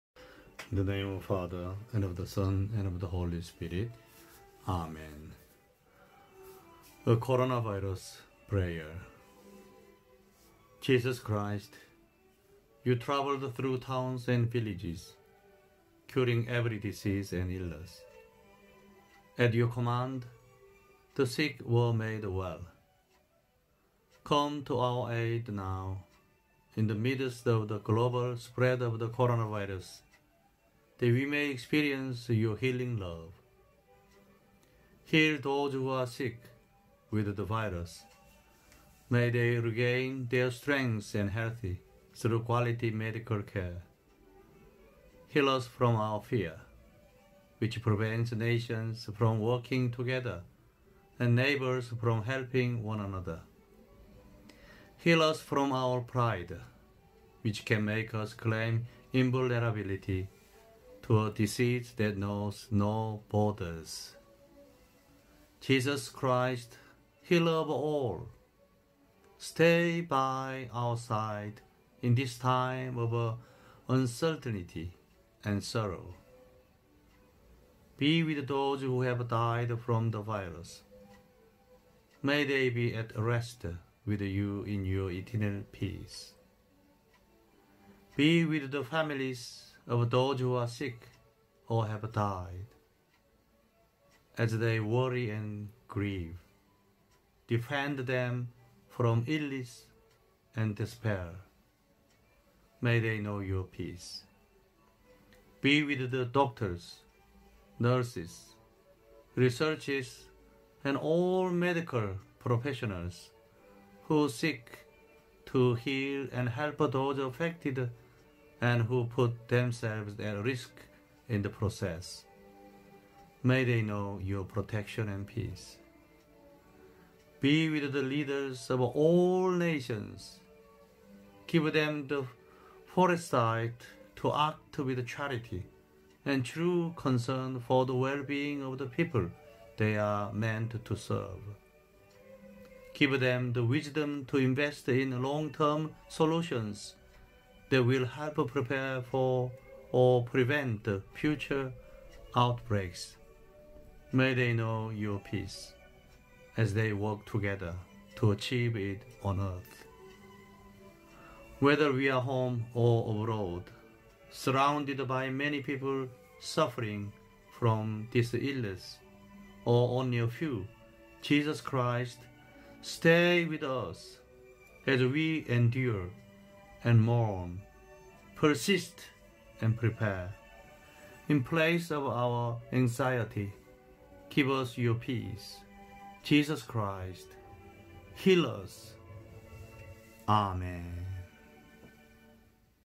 A Coronavirus Prayer 🙏🙏🙏(28/032020)
A Coronavirus Prayer

Jesus Christ, you traveled through towns and villages “curing every disease and illness.” At your command, the sick were made well.

Come to our aid now, in the midst of the global spread of the coronavirus, that we may experience your healing love.

Heal those who are sick with the virus. May they regain their strength and health through quality medical care.

Heal us from our fear, which prevents nations from working together and neighbors from helping one another.

Heal us from our pride, which can make us claim invulnerability to a disease that knows no borders.

Jesus Christ, healer of all, stay by our side in this time of uncertainty and sorrow.

Be with those who have died from the virus. May they be at rest with you in your eternal peace.

Be with the families of those who are sick or have died. As they worry and grieve, defend them from illness and despair. May they know your peace.

Be with the doctors, nurses, researchers and all medical professionals who seek to heal and help those affected and who put themselves at risk in the process. May they know your protection and peace.

Be with the leaders of all nations. Give them the foresight to act with charity and true concern for the well-being of the people they are meant to serve.
Give them the wisdom to invest in long-term solutions that will help prepare for or prevent future outbreaks. May they know your peace, as they work together to achieve it on earth.

Whether we are home or abroad, surrounded by many people suffering from this illness or only a few, Jesus Christ, stay with us as we endure and mourn, persist and prepare. In place of our anxiety, give us your peace. Jesus Christ, heal us.
Amen!